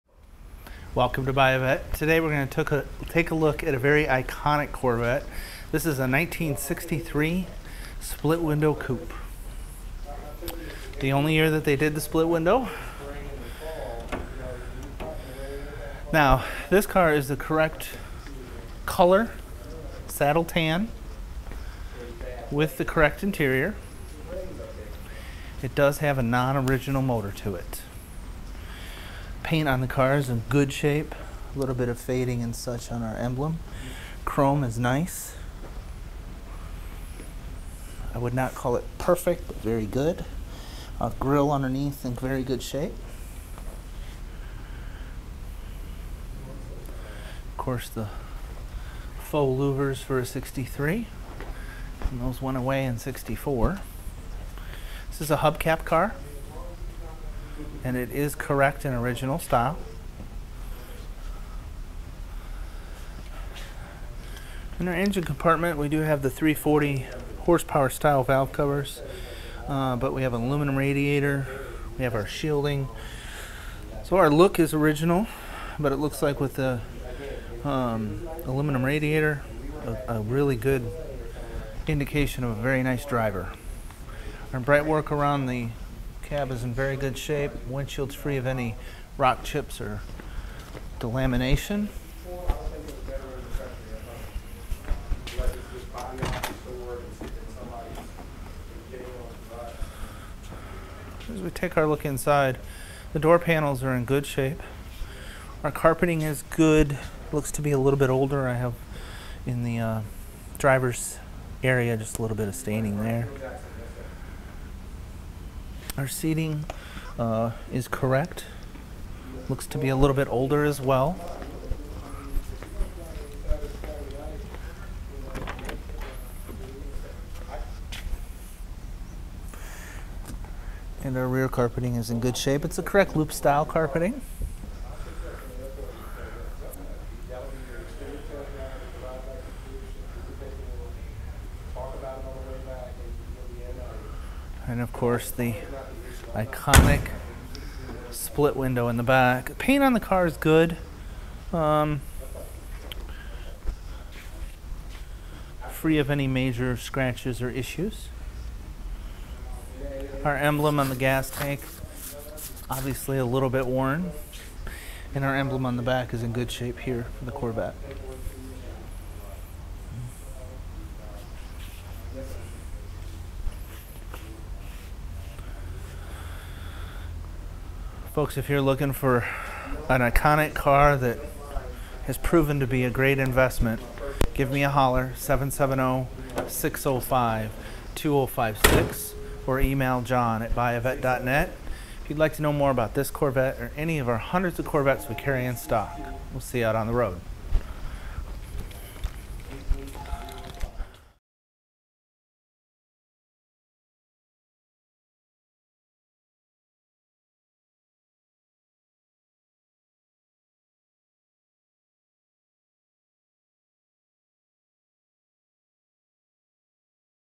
1963 Corvette Split Window Coupe
1963 Corvette Split Window Coupe For Sale

Code correct Saddle Tan exterior, code correct Saddle leather interior.

NOM 327ci engine, 4 speed manual transmission.

Features include genuine leather seats, AM/FM radio.

Originally assembled on December 1, 1962, this 1963 Corvette Split Window Coupe is in good condition, and is sure to turn heads. This car's paint looks to be older, and is in overall good condition with a nice shine. Chrome bumpers show an older or original finish, and are in good condition. Correct style hubcaps are in place, and appear to be original, and steel wheels are in good condition. Older Firestone Supreme SI radial whitewall tires show an average of 9/32nds tread depth remaining. The interior of this 1963 Corvette Split Window Coupe shows older leather seats in good condition, along with good-vg carpet. Gauges are avg. Correct appearing AM/FM radio is in place. The NOM engine in this car shows correct casting number 3782870, and was cast on December 29, 1964. Correct ignition shielding, radiator hoses, and hose clamps are in place. Additions to this engine include a chrome air cleaner, Edelbrock carburetor, 340hp style valve covers, and an aluminum radiator. Frame is solid and rust-free.